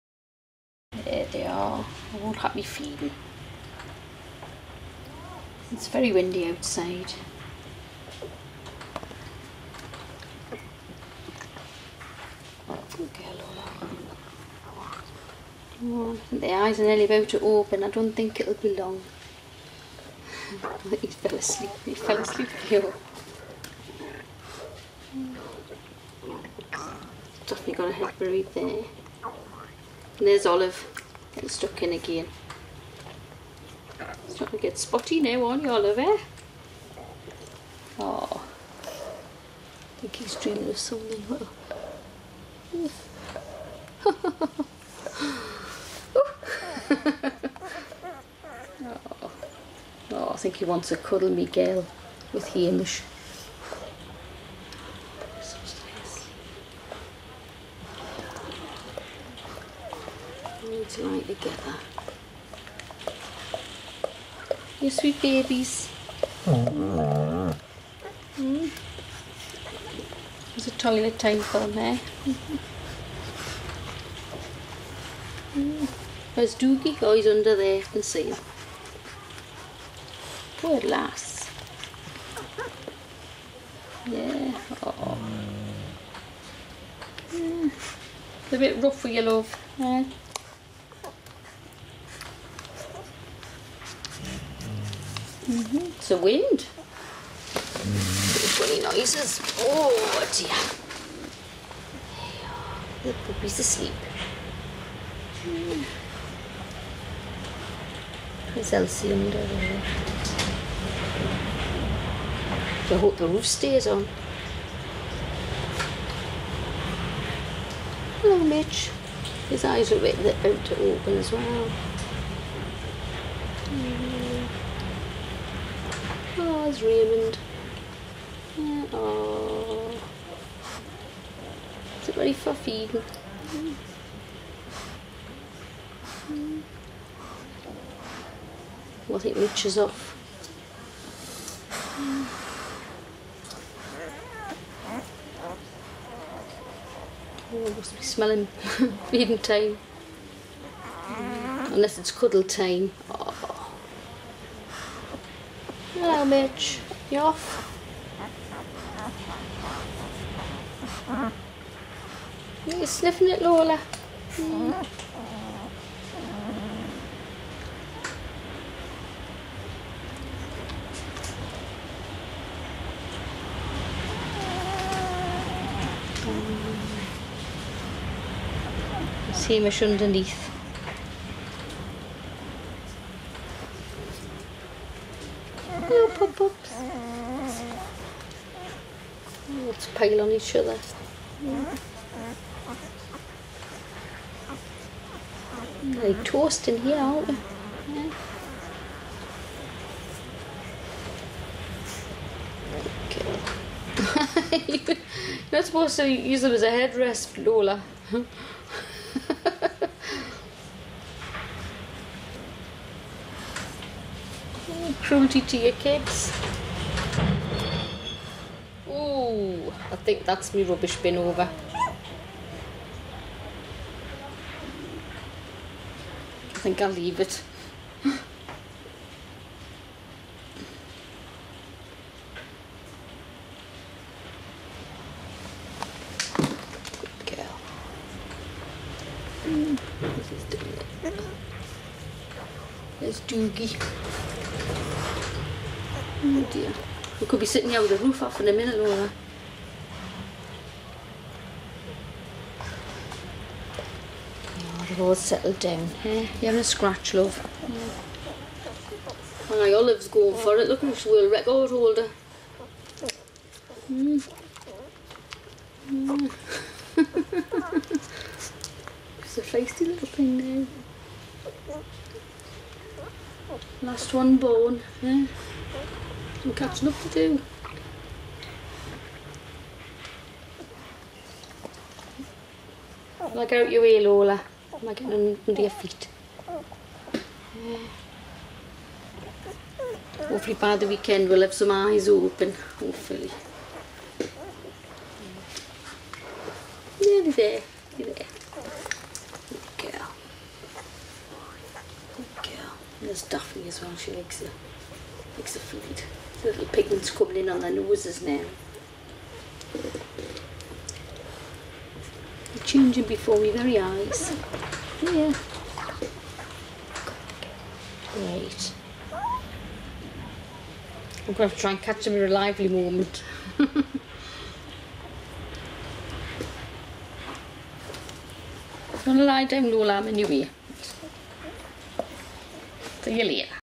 Lola's second litter (9 days old)
Puppies eyes nearly open it won't be long